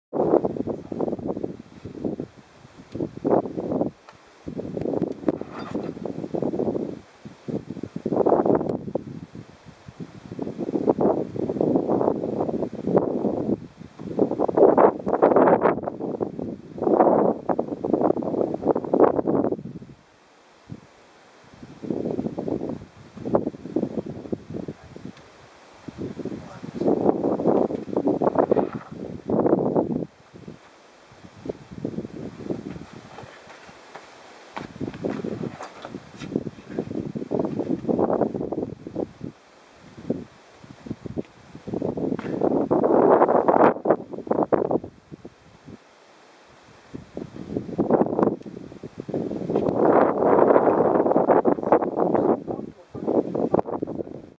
Bishop Bouldering - Mr Happy V5 - Joey
Joey onsites Mr. Happy.